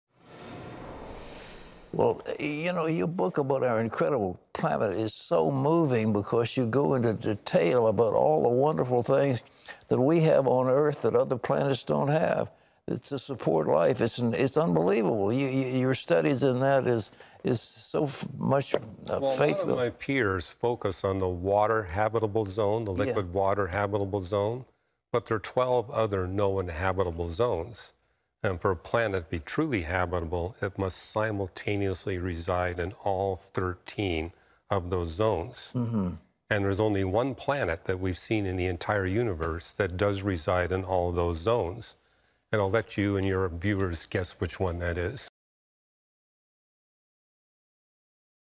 How Special is the Earth’s Ability to Support Life?
Astrophysicist Hugh Ross shares what makes Earth's unique in its ability to support life, and whether he believes there are other planets just like ours.
►BUY Hugh Ross's book, "Lights in the Sky & Little Green Men"